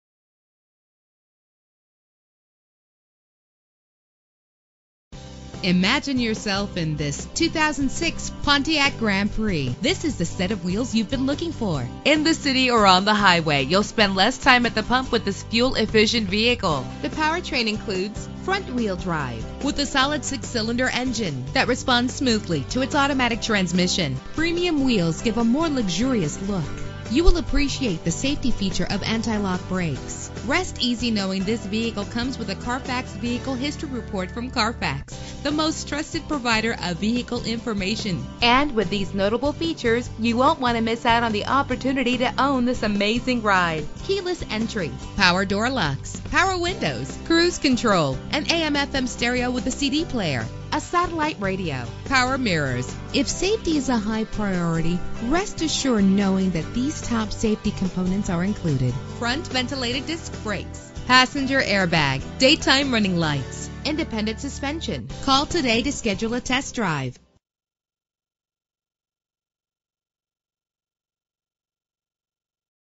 2006 Pontiac Grand Prix - Hillsboro OR
Year: 2006
Make: Pontiac
Model: Grand Prix
Trim:
Engine: 3.8 liter 6 cylinder 12 valve
Transmission: 4-Speed Automatic
Color: White
Mileage: 121975
Address:
4151 S. E. Tualatin Valley Highway